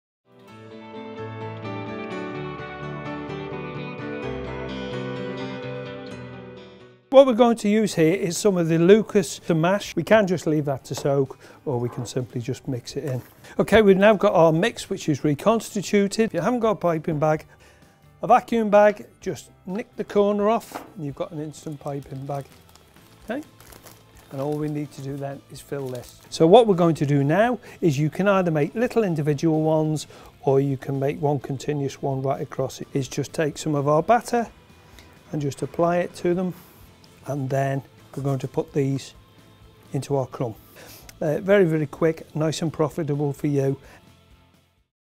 Potato Croquettes
Sauce-It from Dalziel with Verstegen show simple potato croquettes to help give a meal solution for your customers.